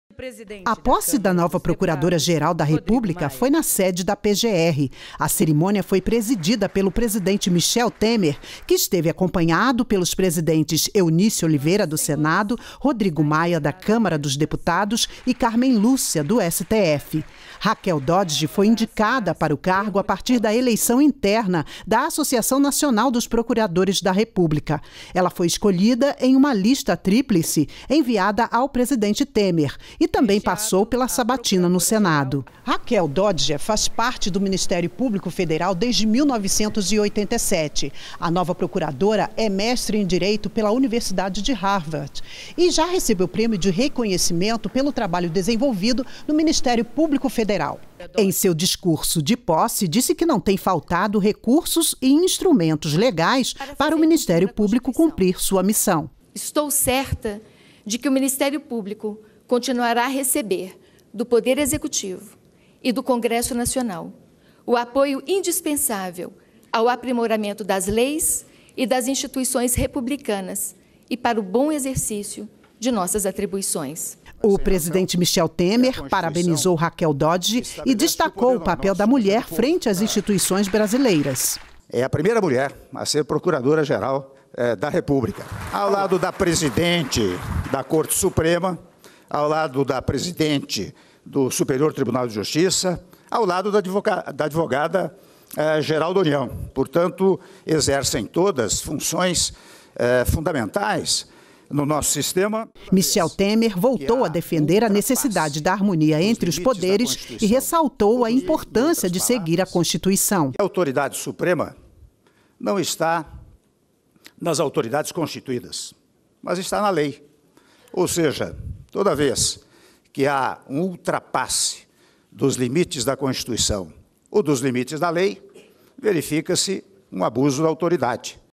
Raquel Dodge ocupa o cargo de procuradora-geral da República
REPÓRTER NBR - 18.09.17: Raquel Dodge tomou posse, nesta segunda-feira (18) pela manhã, como a primeira mulher a ocupar o cargo de procuradora-geral da República. Raquel substitui Rodrigo Janot, que deixou o cargo após quatro anos à frente do Ministério Público Federal. Raquel Dodge ficará dois anos no cargo e poderá ser reeleita pelo mesmo período.